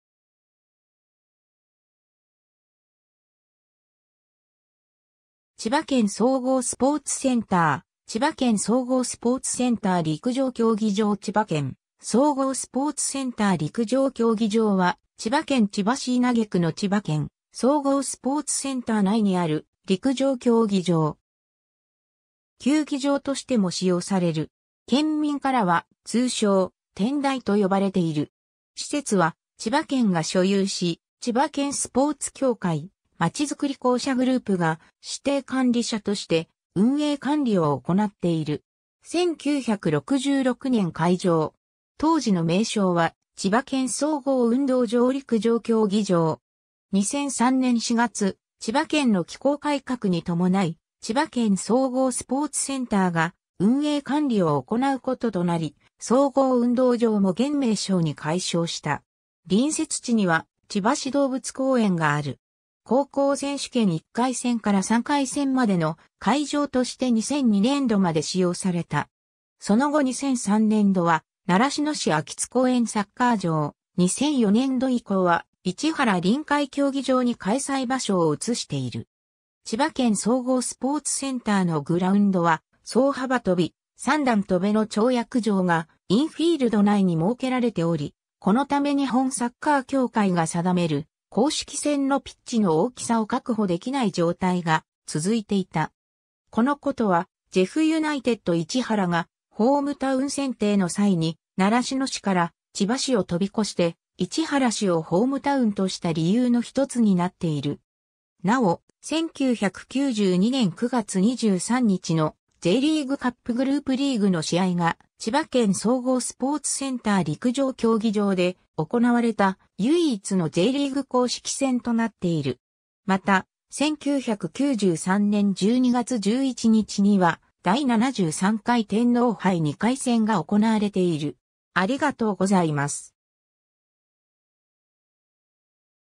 千葉県総合スポーツセンター陸上競技場
千葉県総合スポーツセンター 」 千葉県総合スポーツセンター陸上競技場 千葉県総合スポーツセンター陸上競技場（ちばけんそうごうスポーツセンターりくじょうきょうぎじょう）は、千葉県千葉市稲毛区の千葉県総合スポーツセンター内にある陸上競技場。
県民からは通称「天台」と呼ばれている。
施設は千葉県が所有し、千葉県スポーツ協会・まちづくり公社グループが指定管理者として運営管理を行っている。
 1966年開場。
当時の名称は千葉県総合運動場陸上競技場（ちばけん・そうごううんどうじょう・りくじょうきょうぎじょう）。
2003年4月、千葉県の機構改革に伴い千葉県総合スポーツセンターが運営管理を行うこととなり、総合運動場も現名称に改称した。
 隣接地には千葉市動物公園がある。
 高校選手権1回戦から3回戦までの会場として（西暦奇数年度の大会によっては、準々決勝まで使われたことがある）2002年度まで使用された。
その後2003年度は習志野市秋津公園サッカー場、2004年度以降は市原臨海競技場に開催場所を移している。
 千葉県総合スポーツセンターのグラウンドは走幅跳、三段跳の跳躍場がインフィールド内に設けられており、このため日本サッカー協会が定める公式戦のピッチの大きさを確保できない状態が続いていた。
この事はジェフユナイテッド市原（当時）がホームタウン選定の際に習志野市から千葉市を跳び越して市原市をホームタウンとした理由の一つになっている（現在は千葉市・市原市の広域ホームタウンであるが、千葉市での試合会場は本スタジアムではなく、フクダ電子アリーナである）。
 なお、1992年9月23日(水)の、Jリーグカップグループリーグの試合（東日本JR古河×読売クラブ）が、千葉県総合スポーツセンター陸上競技場で行われた唯一のJリーグ公式戦となっている。
（パヴェルの延長サドンデスゴールによりホームの東日本JR古河が 1-0 で勝利。） また、1993年12月11日(土)には、第73回天皇杯2回戦（ジェフユナイテッド市原 2-0 東芝）が行われている。